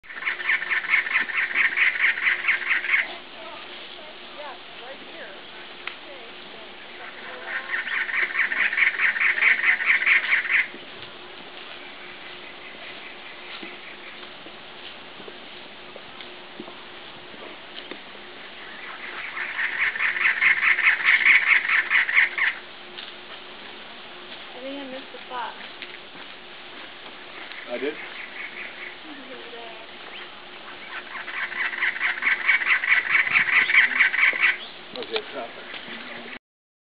Right after the rains ended at the Arizona-Sonora Desert Museum, this cactus wren began his throaty serenade. 2/6/10